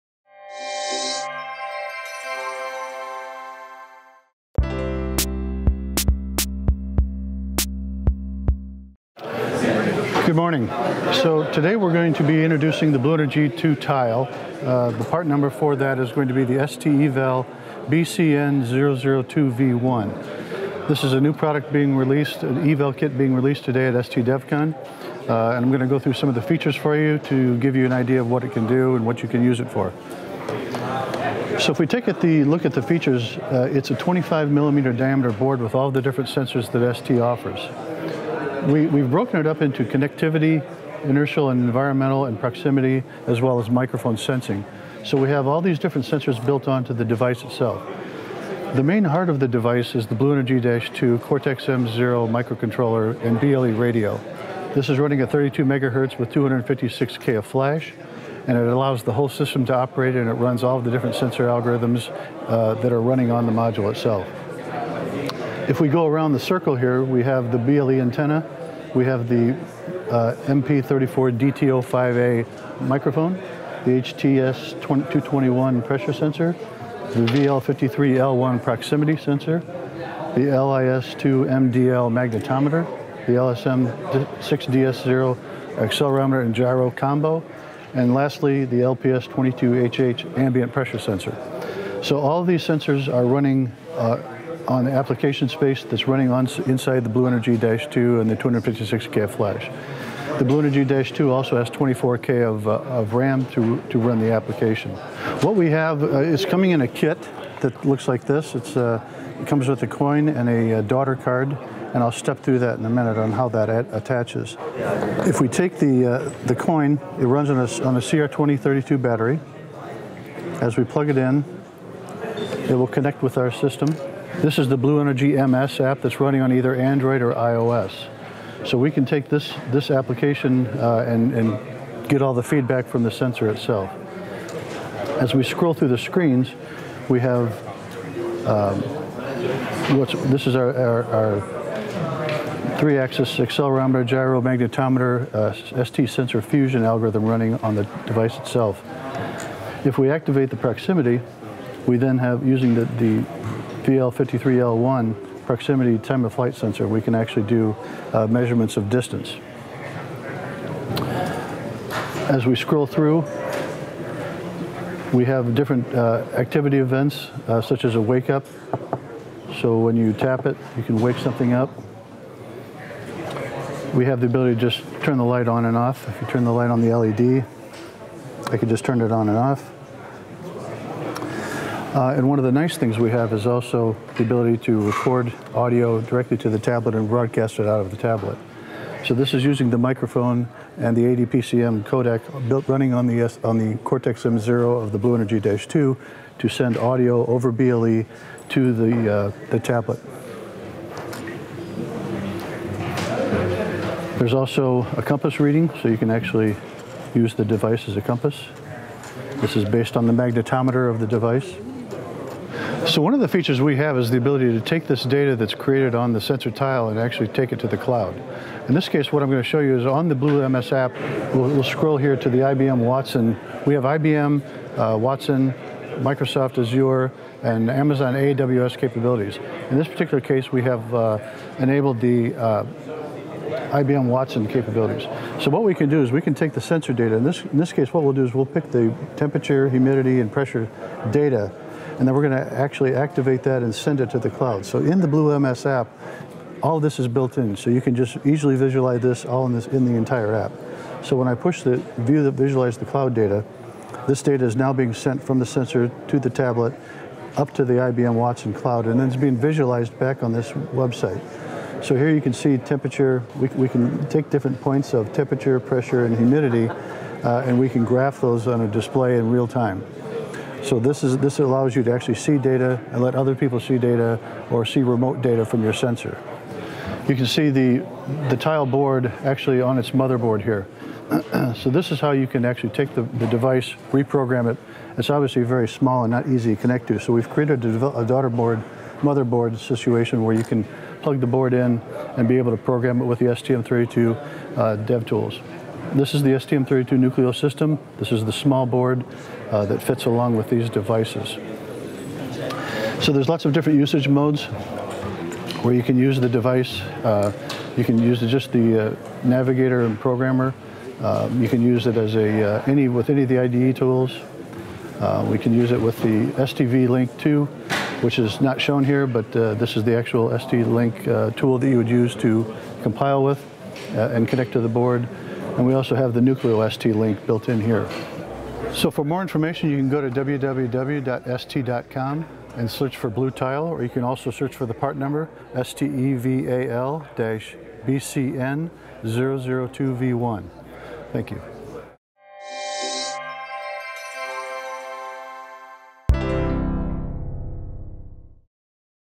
ST DevCon 2018: BlueNRG Tile
The STEVAL-BCN002V1B Bluetooth LE enabled sensor node development kit features the STEVAL-BCN002V1 multi-sensor board based on BlueNRG-2 SoC Bluetooth Low Energy application processor. This sensor board has accelerometer, gyroscope, magnetometer, pressure, humidity, Time-of-Flight and microphone sensors, and is powered by a common CR2032 coin battery.

The STEVAL-BCN002V1D adapter board is used to program and debug the sensor board. The adapter board is powered via USB.